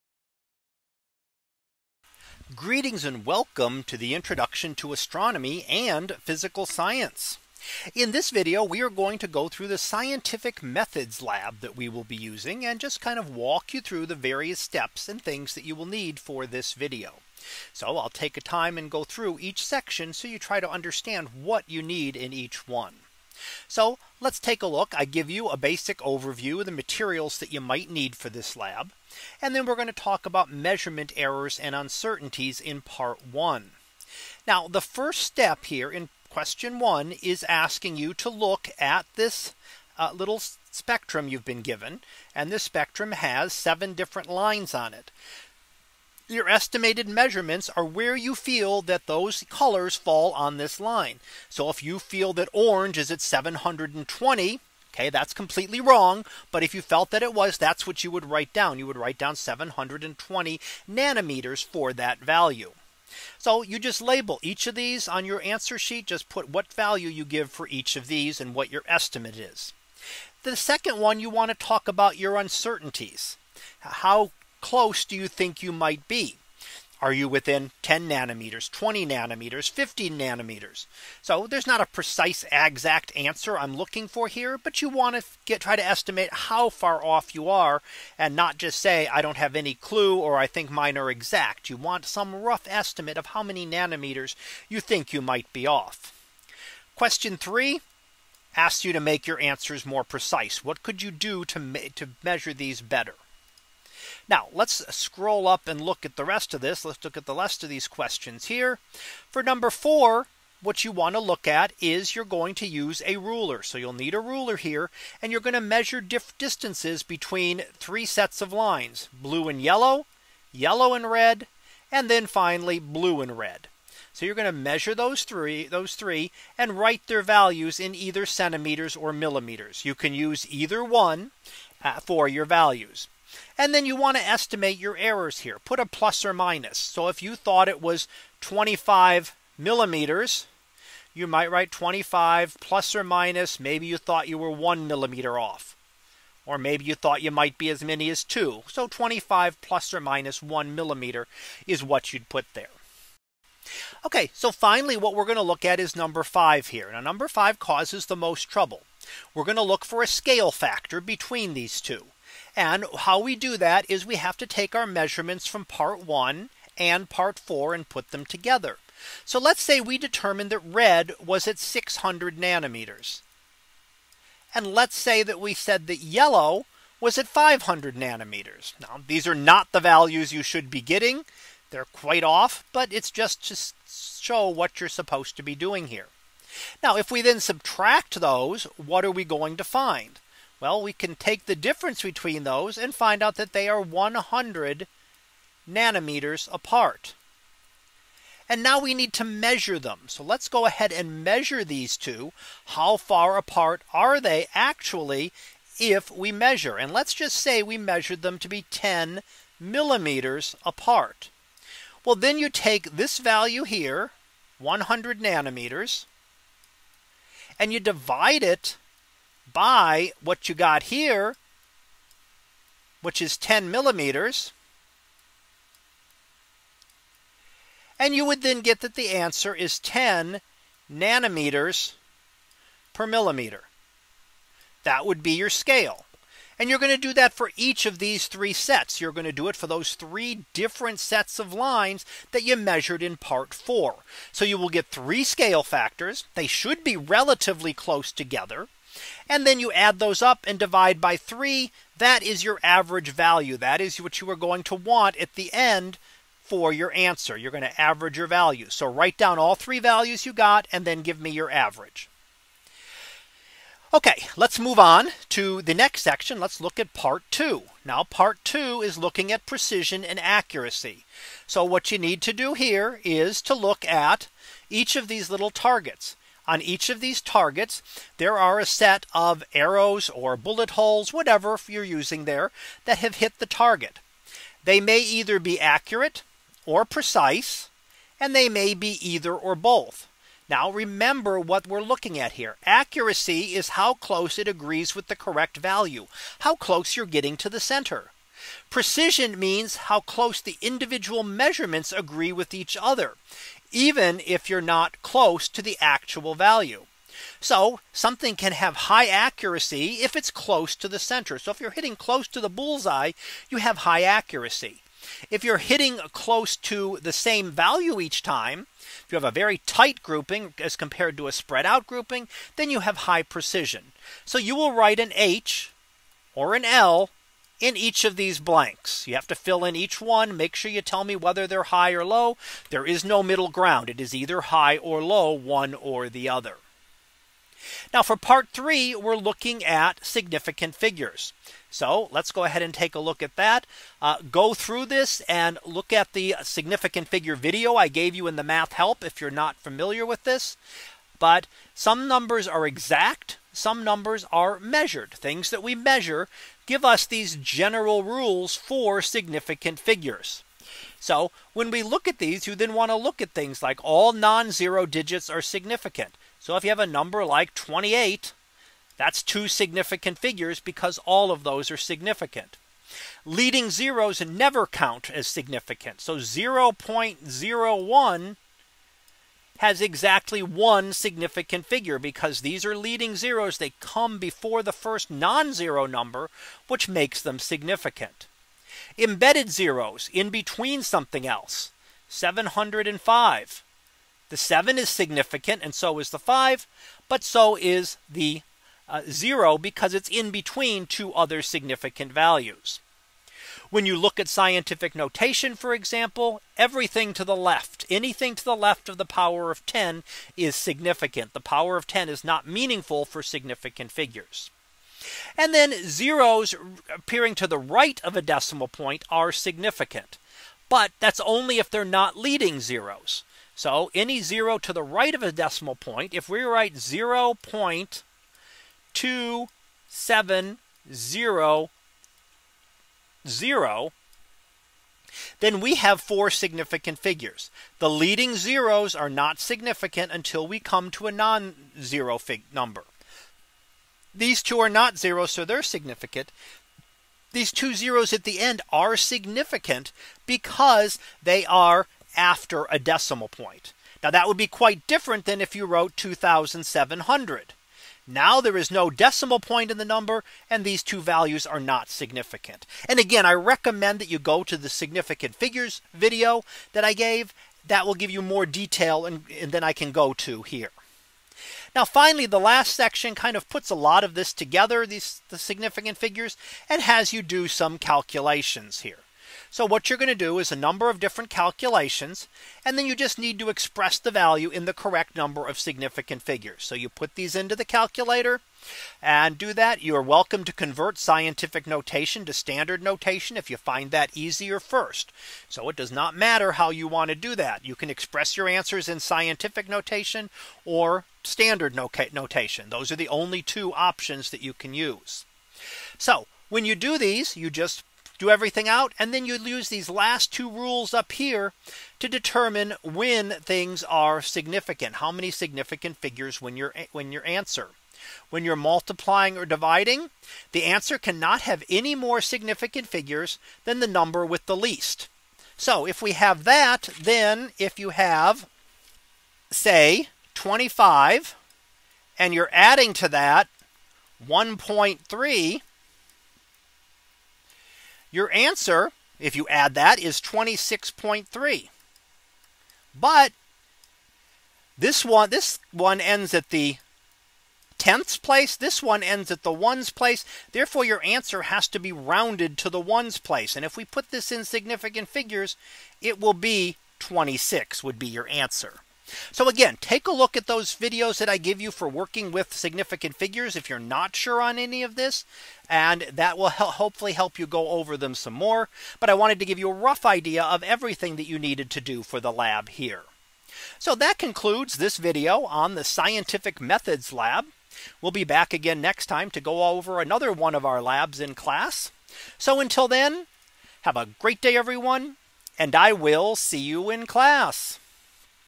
Physical Science Lab - Scientific Methods
In this video, I will discuss the Scientific Methods lab that we sometimes do in my astronomy and physical science classes.